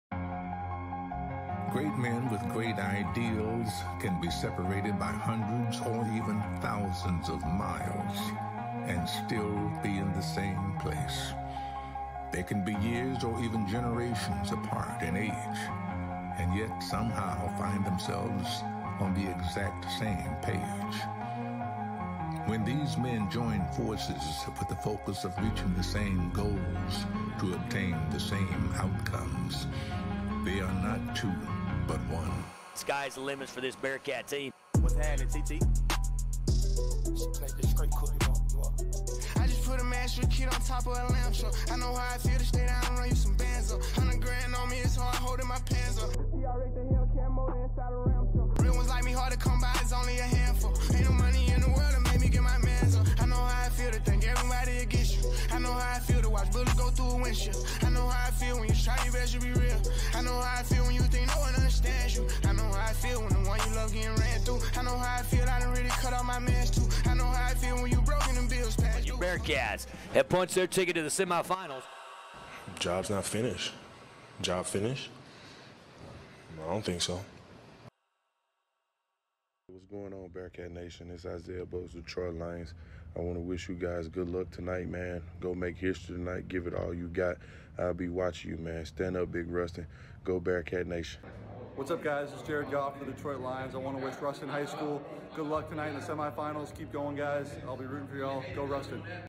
Ruston Bearcats Hype Video
As Ruston is the #1 team in Louisiana and also attending Ruston High School I made something a little special. Ruston will take on Destrehan, 12/9/22 at the Super Dome in New Orleans as they advanced in the Semifinals against Zachary. I also included a special message from Isiah Buggs and Jared Goff as Isiah Buggs used to attend at Ruston High School.